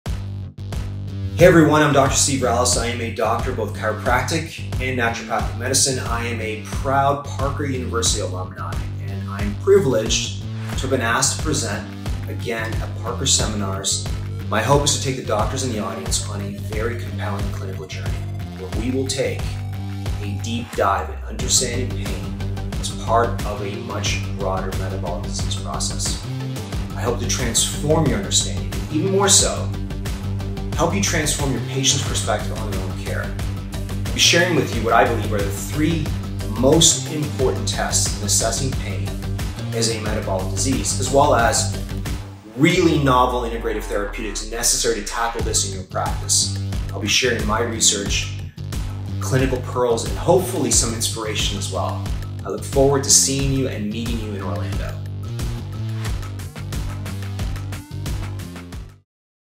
Steve Rallis - Parker Seminars Orlando 2024
Dr. Steve Rallis is a licensed doctor of both chiropractic and naturopathic medicine and adjunct faculty at Trinity Medical School in Integrative Medicine. He received his undergraduate degree in biological sciences from Western University in London, Ontario, his chiropractic degree from Parker University in Dallas, Texas where he received a research scholarship and his naturopathic medical degree from the Canadian College of Naturopathic Medicine. With a keen interest in both research and education, his private practice is in Barrie, Ontario and is one of the world’s first to offer integrative/functional medicine rotations for medical students. His published and ongoing research is focused on the integrative treatment of cancer and metabolic diseases including cancer as a metabolic process. He is an author and international lecturer to patients and physician groups throughout North America, South America, and Europe. Dr. Rallis is a Parker University alumni board member.